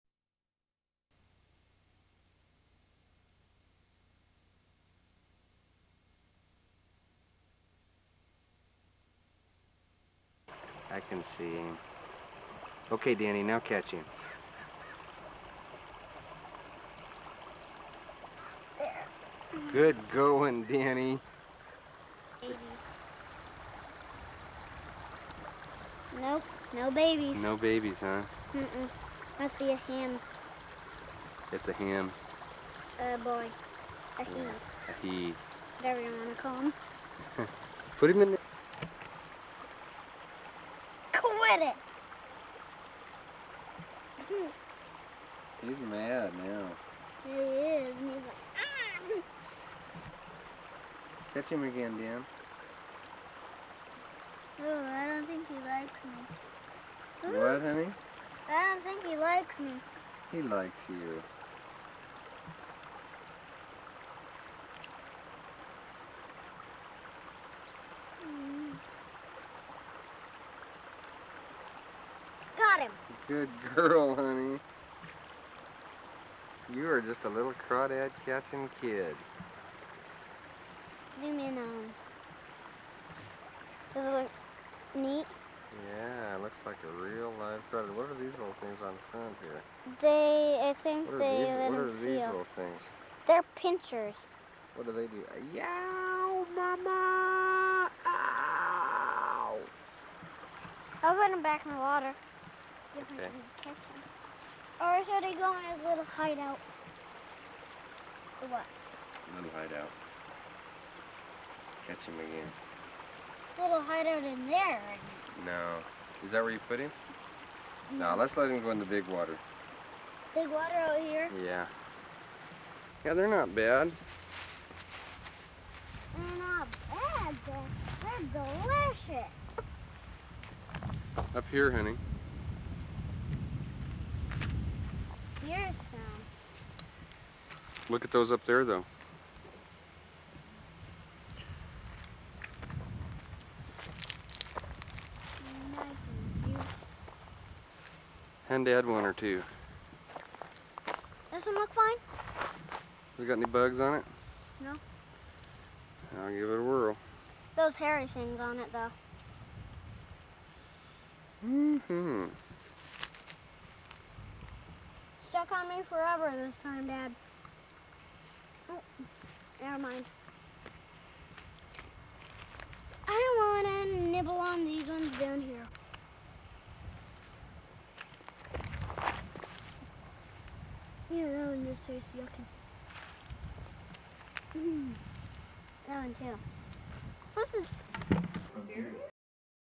Dani Catching Crawdads
My daughter, the little Tomboy, catching crawdaddies in the creek near our house way back in 1990. Then she "Nibbles" on some Blackberries.